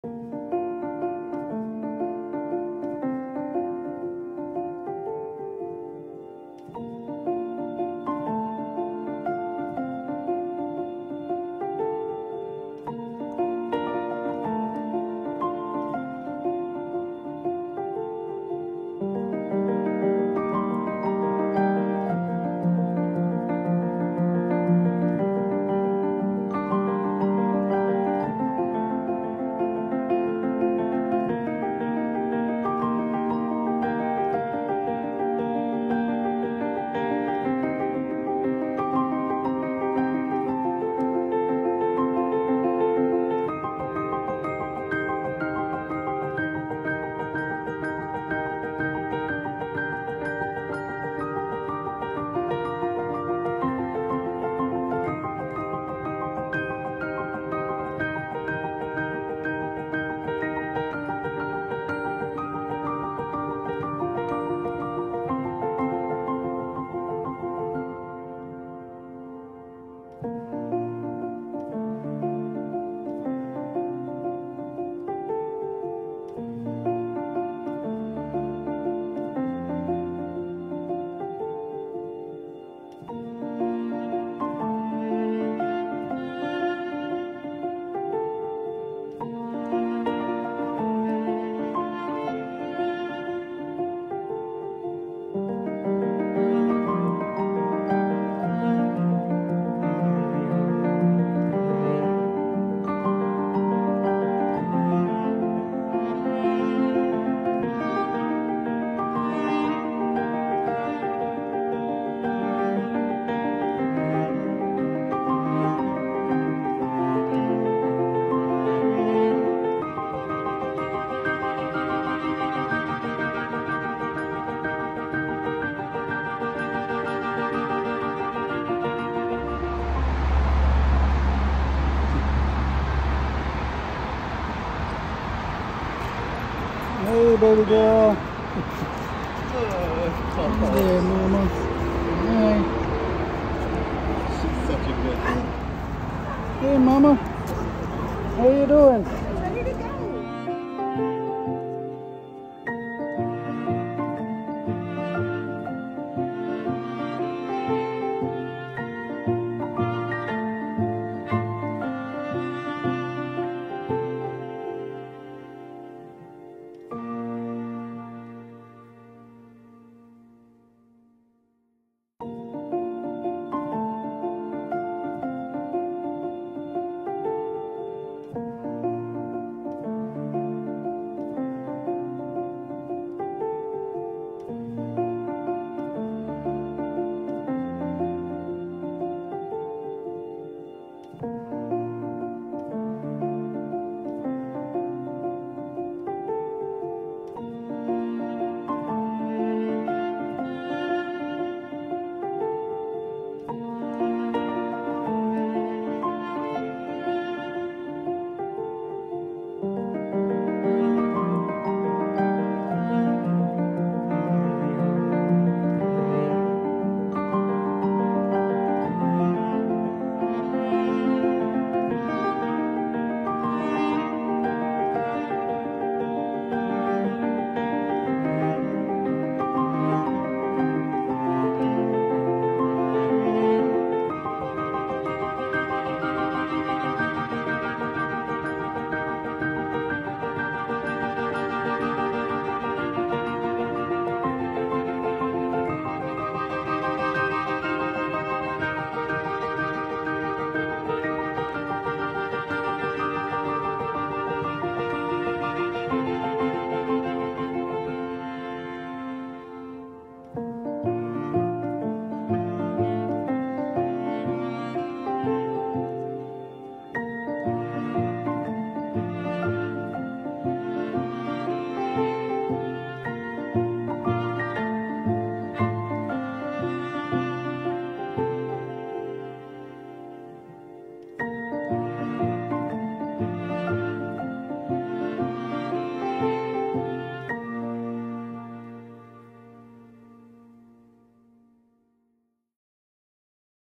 Best Chocolate Lab. Tribute to Sweet Bailey.
Lost my sweet girl after her battle with nasal chondrosarcoma. I had her for 12 years. She was so very precious and I am blessed to have had her in my life.